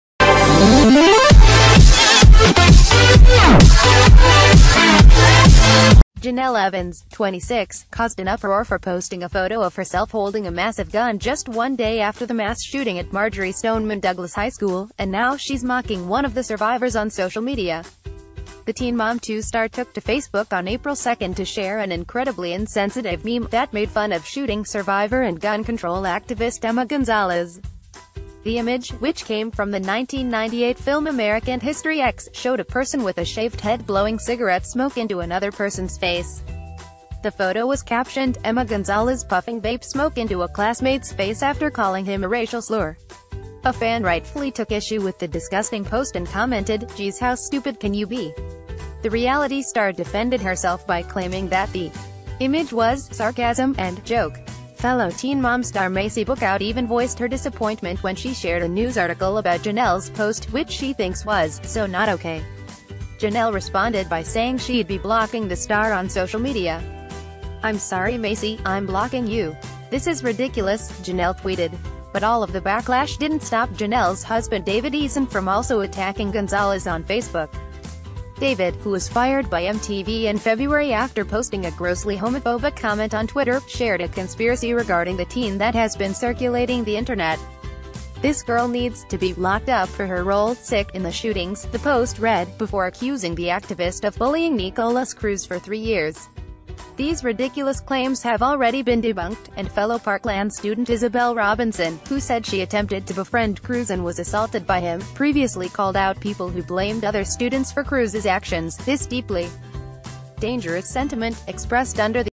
Jenelle Evans & Husband Slam Parkland Shooting Survivor Emma Gonzalez: She Should Be ‘Locked Up’
Jenelle Evans, 26, caused an uproar for posting a photo of herself holding a massive gun just one day after the mass shooting at Marjory Stoneman Douglas High School, and now she’s mocking one of the ...